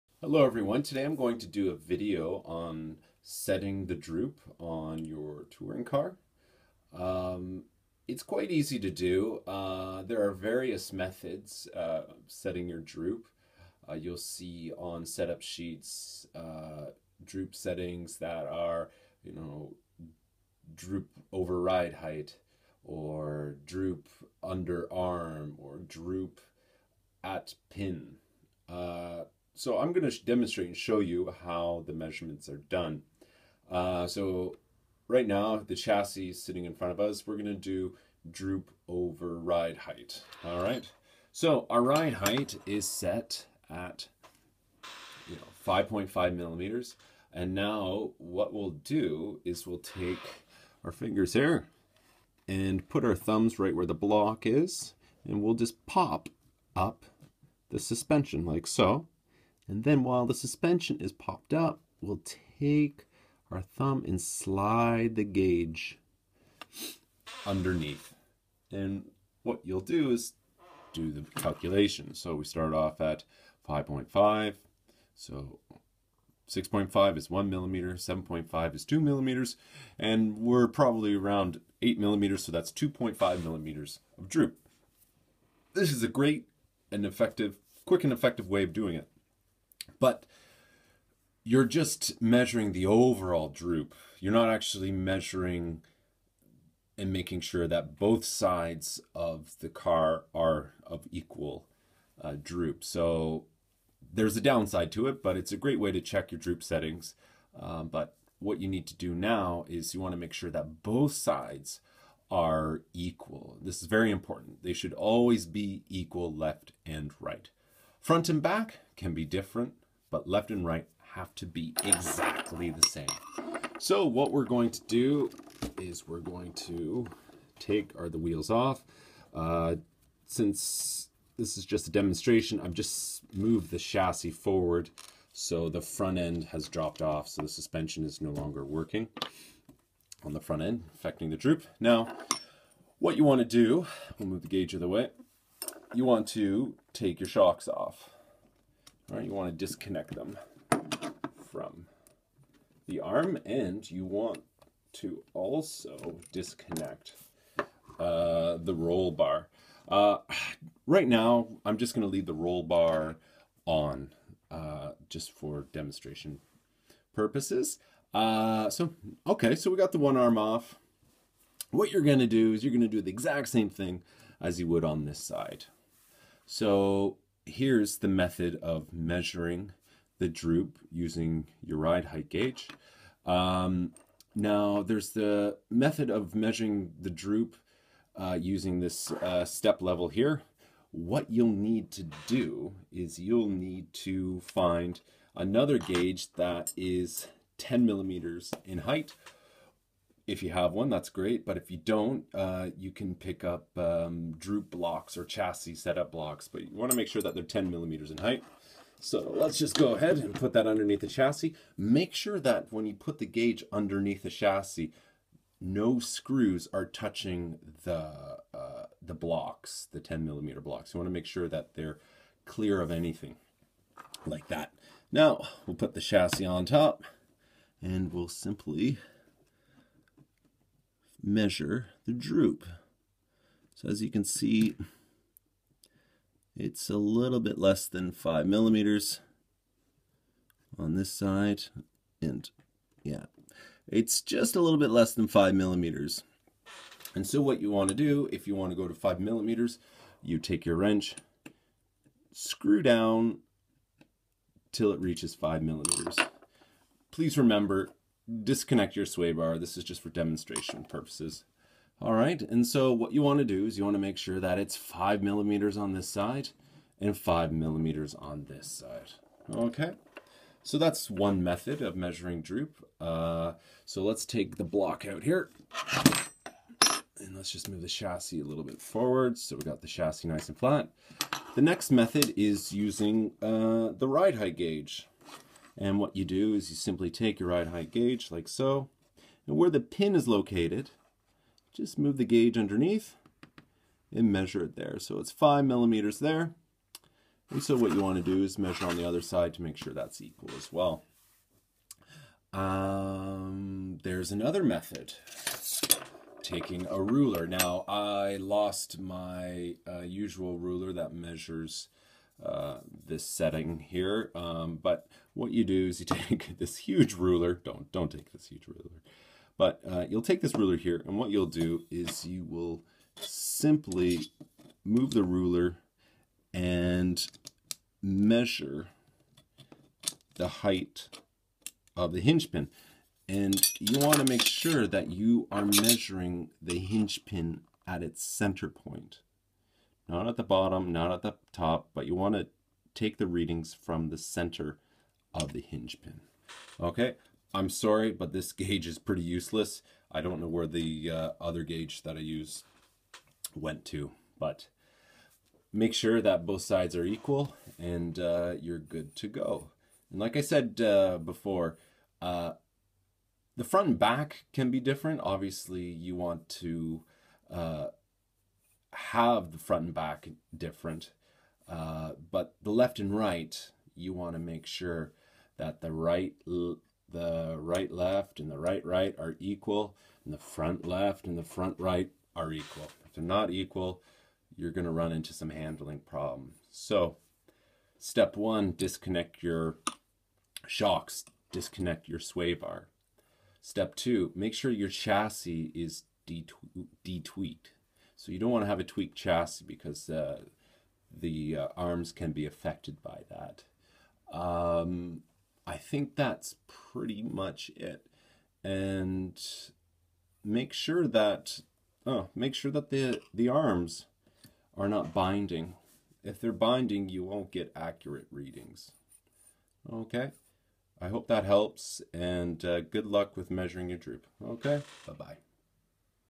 Measuring Droop
Simple video covering different methods of measuring droop.